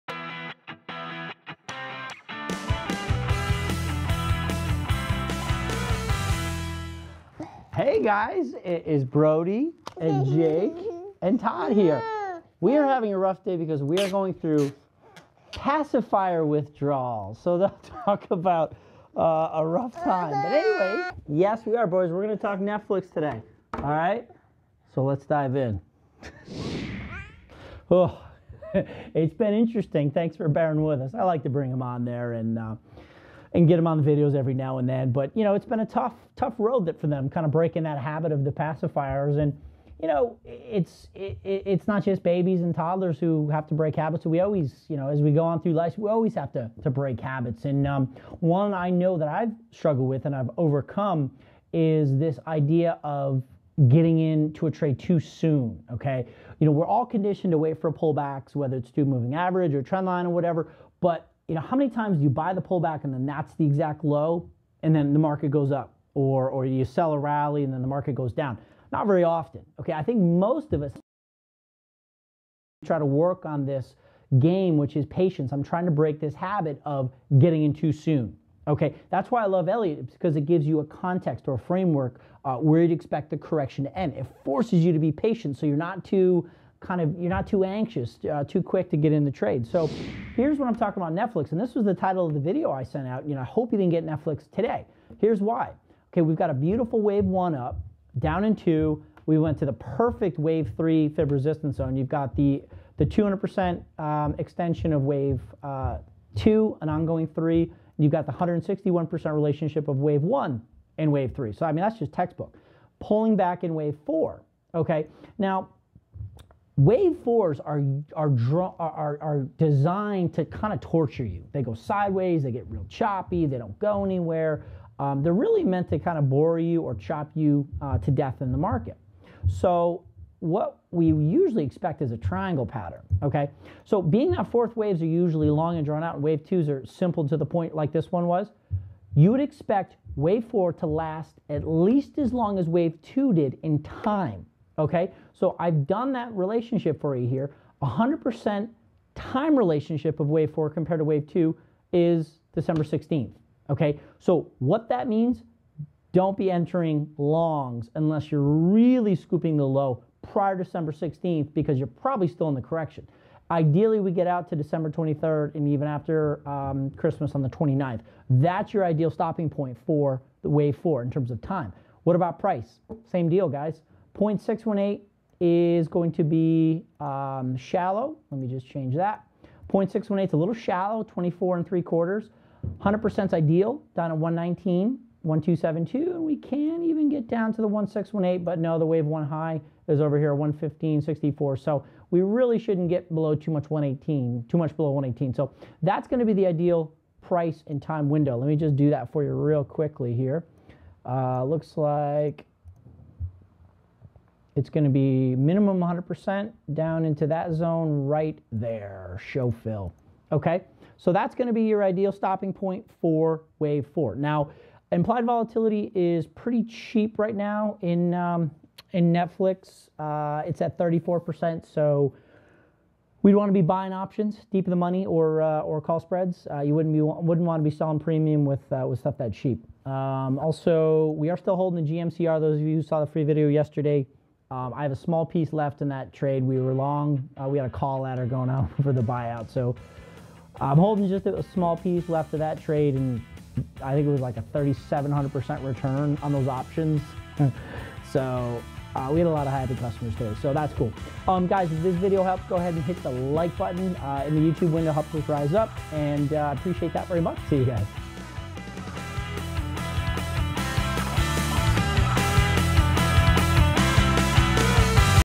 I Hope You Didn't Buy Netflix Today?
Like my twin toddlers, we are always working on our bad habits.  One of those I see consistently is not being patient with entries and letting the market come to us to ensure premium entries.  Let Jake, Brody, and Todd take you through an example of a patient entry in NFLX.